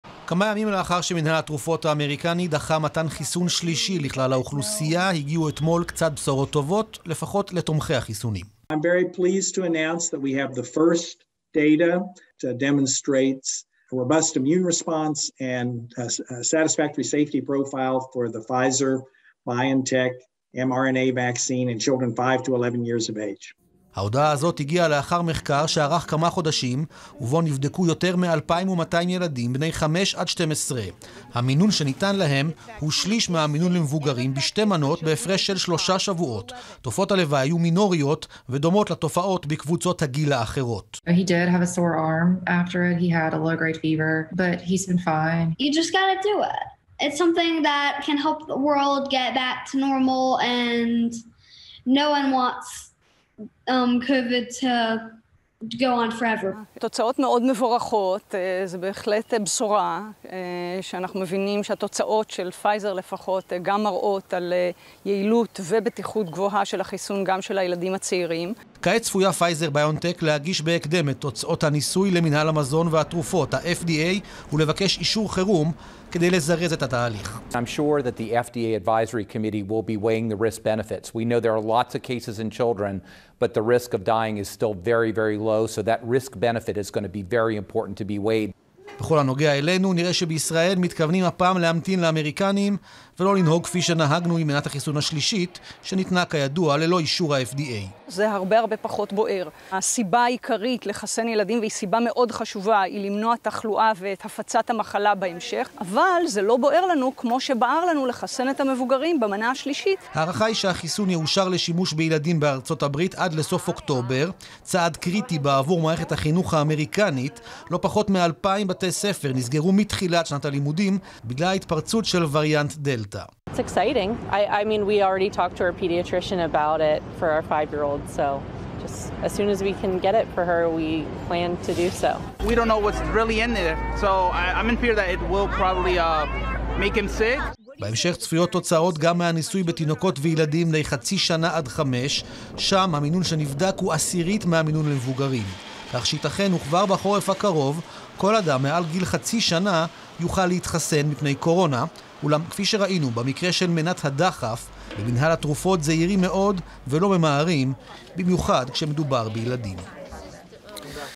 בדרך לחיסון בני 11-5? פייזר הודיעה על ניסוי מוצלח
כאן חדשות | ענקית התרופות הודיעה היום, לאחר ניסיון שביצעה, שכמות פחותה של החיסון - שליש מזו הניתנת לבני נוער ומבוגרים - בטוחה ומעוררת תגובה חיסונית בקרב ילדים. בישראל צפויים להמתין עם ההחלטה לחסן ילדים עד למתן אישור ה-FDA. כתבתו של איתמר מאירי, מתוך חדשות הערב 21.09.21